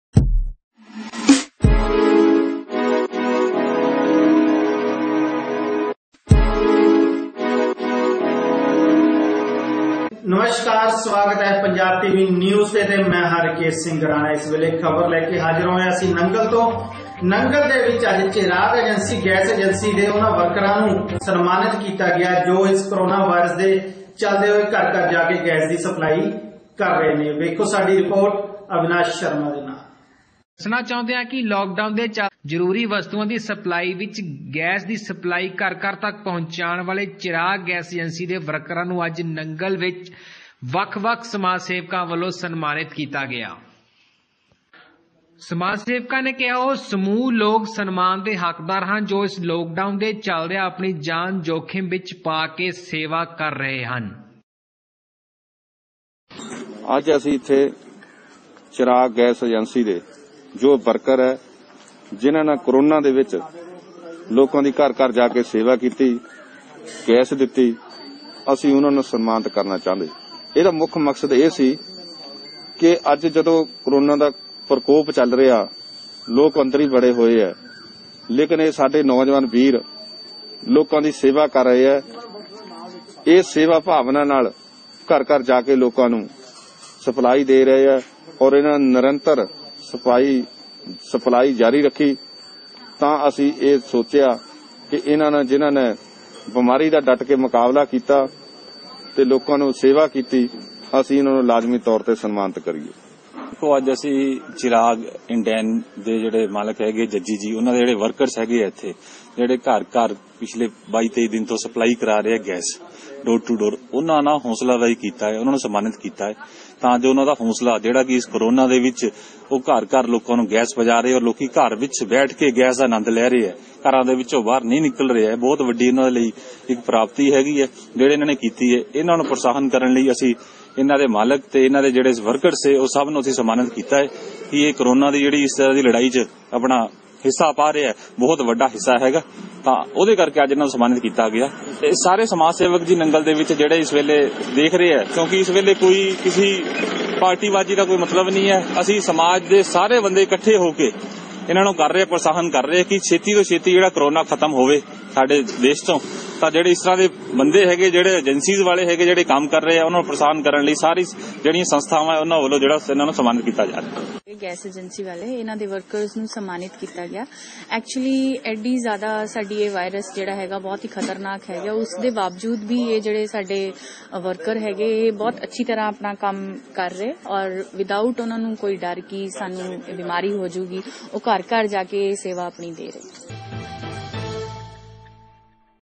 ਚਿਰਾਗ ਗੈਸ ਏਜੰਸੀ ਦੇ soldier ਵੀ ਕੀਤੇ ਗਏ ਸਨਮਾਨਿਤ#gasagency
ਸਮਾਜ ਸੇਵੀ ਸੰਸਥਾ ਨੇ ਕੀਤਾ ਸਨਮਾਨਿਤ#ptv